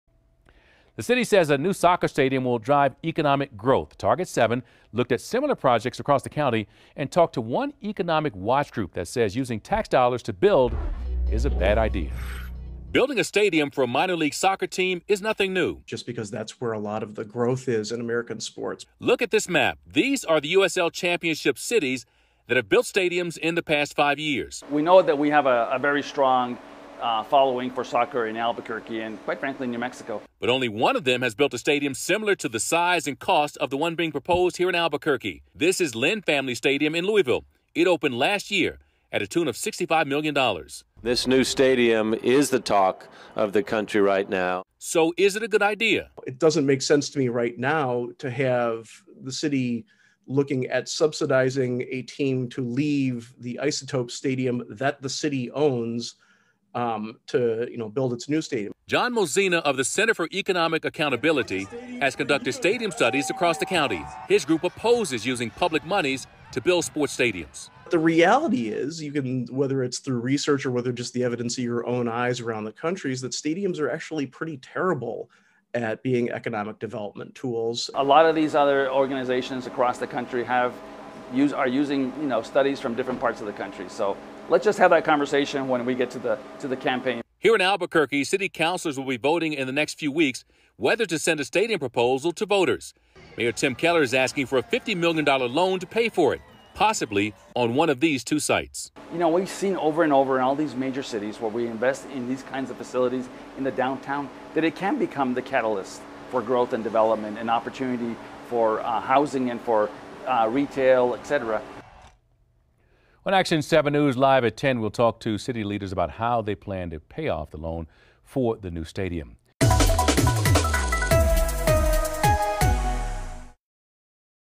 Ten other USL cities have recently built stadiums
Economic watch group says using tax dollars to pay for stadium is bad idea